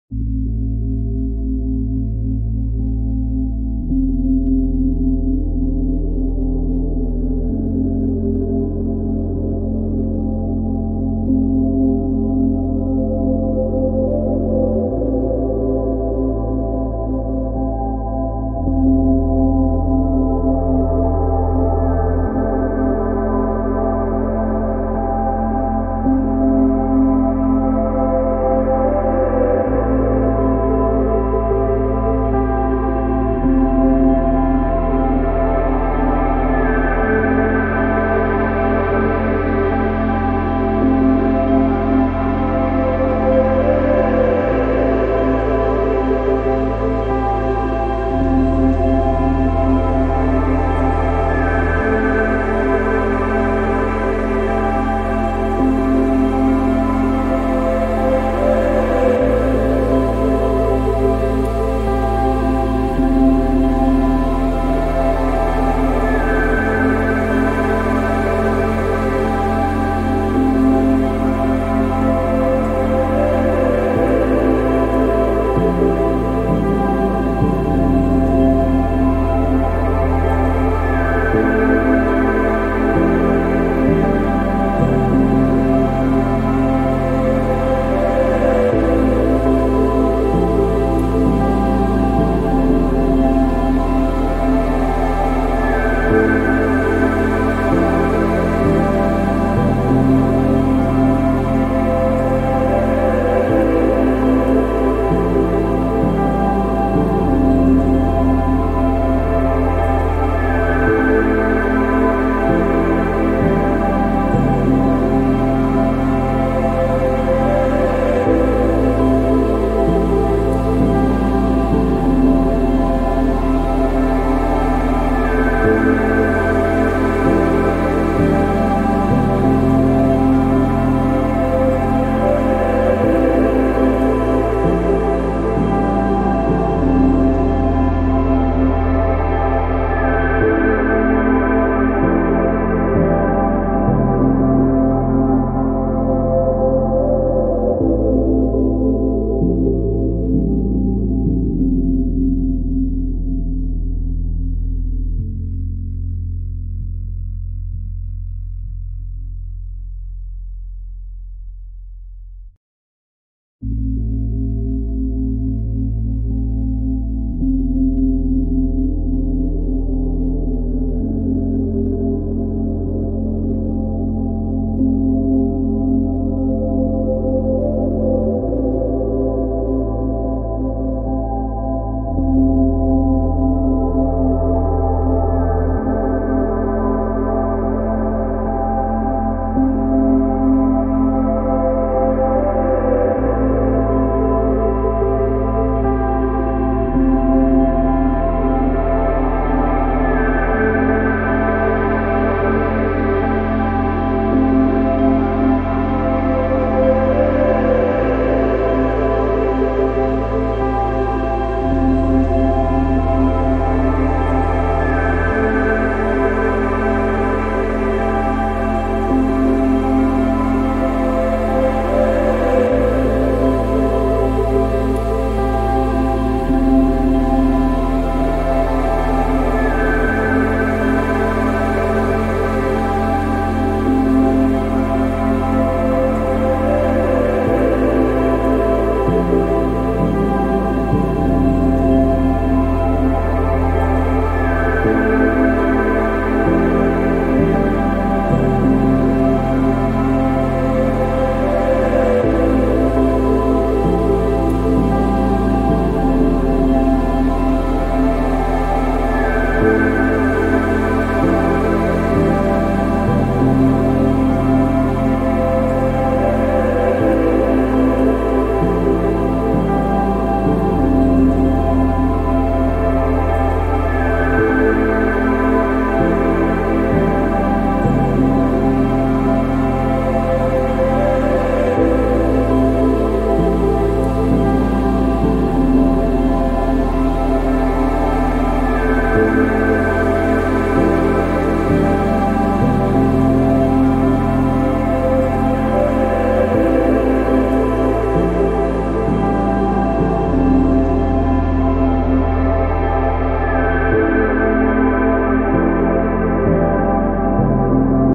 Forest Relaxation music/Meditation Deep Relaxing music#rksinghmeditation
This video is soothing relaxing music and Relaxing meditation music and amazing Water Fall create by RK Singh Meditation